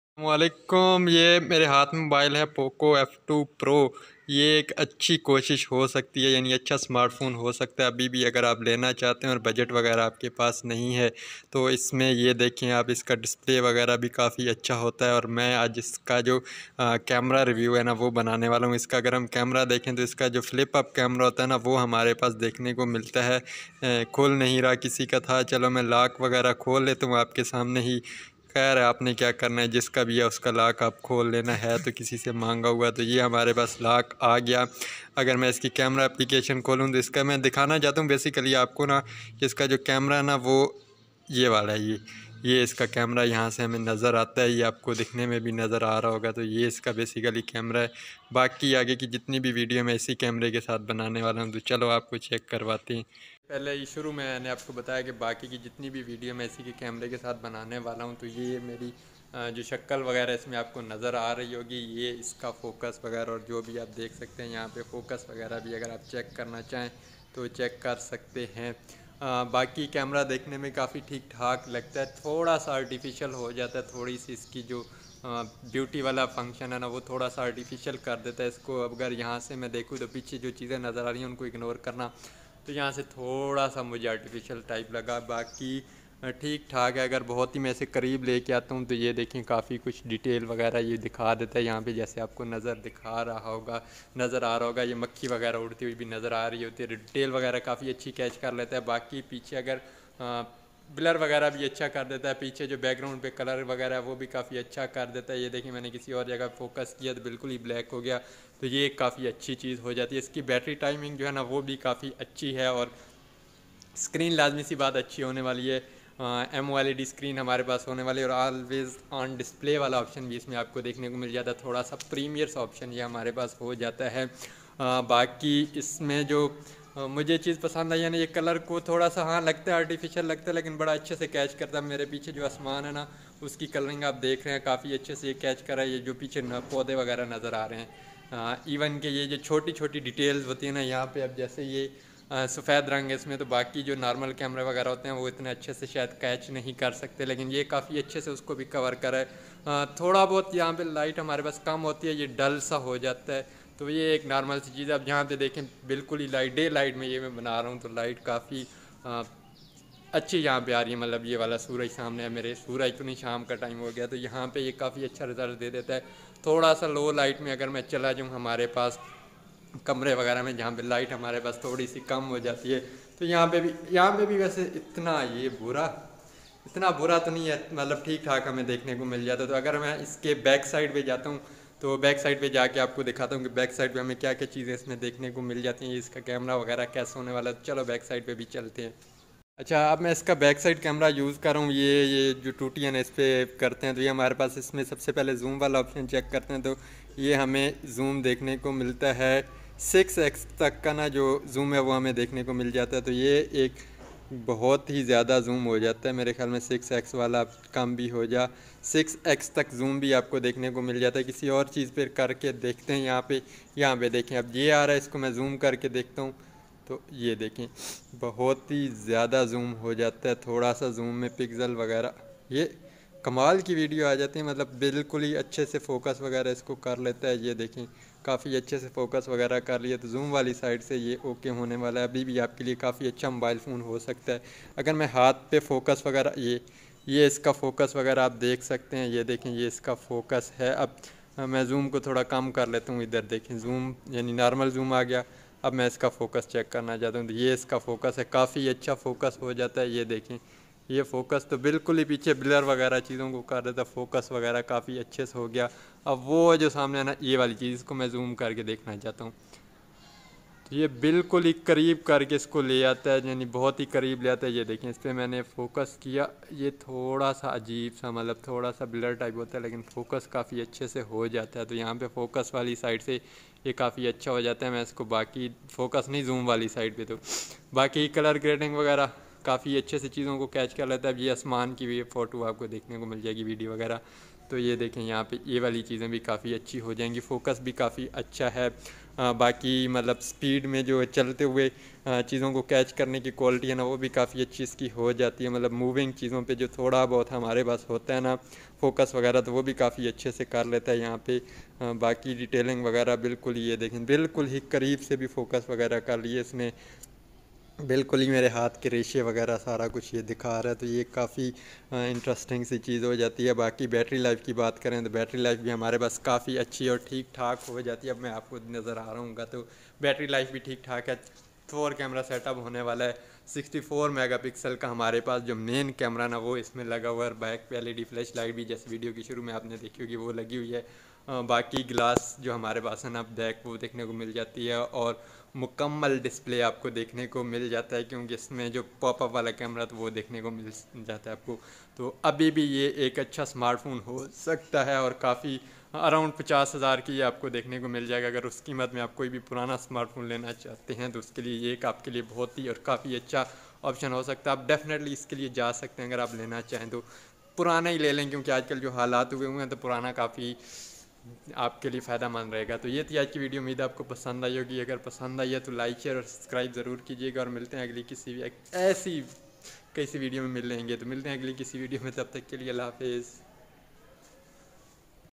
Best smart phone under 45k in Pakistan- Poco F2 pro| Poco F2 Camara Review
in this video I'm going to talk about
Best smart phones under 45k in Pakistan- Poco F2 pro| Poco F2 Camara Review|